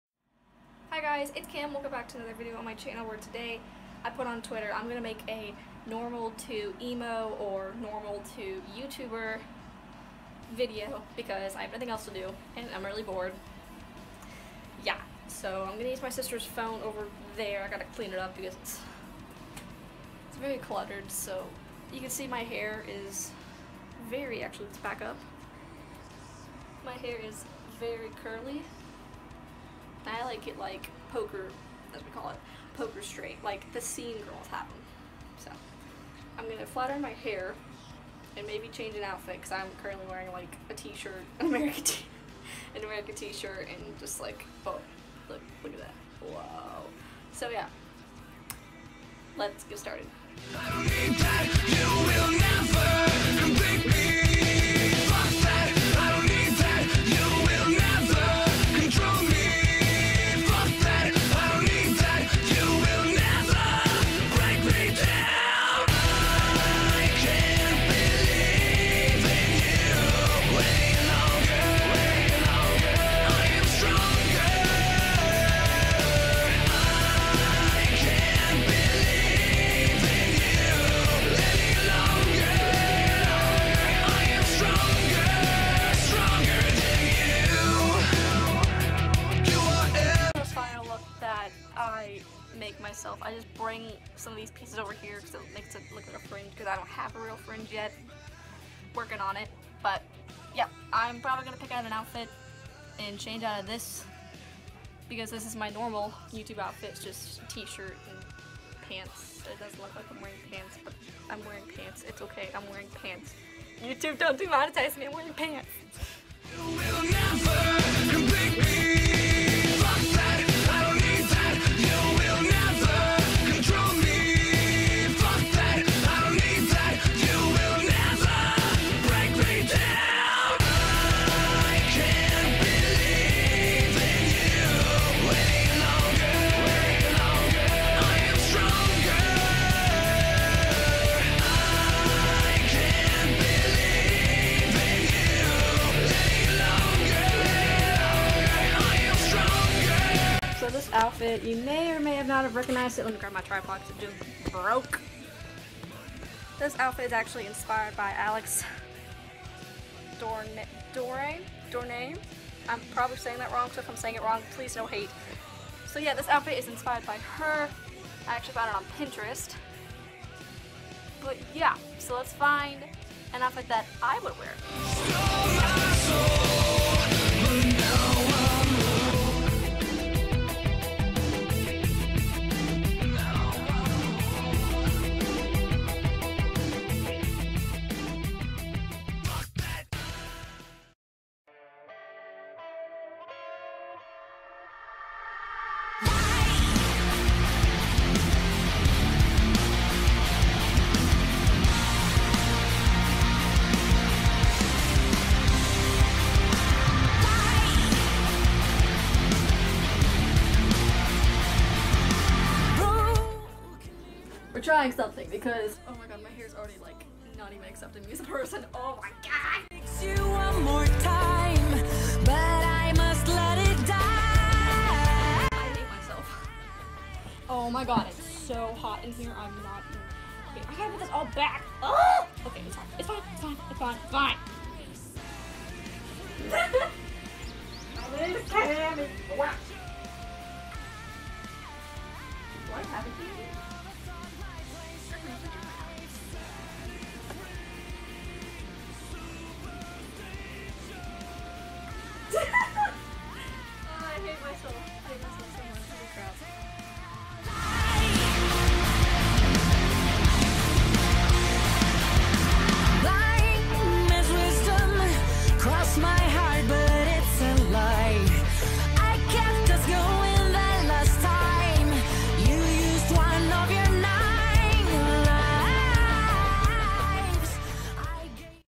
Normal to Scene (might delete later)
so this is a very quick video i did of me trying to be scene

you can hate in your minds just dont hate in the comments

thanks XD

low key proud of the thumbnail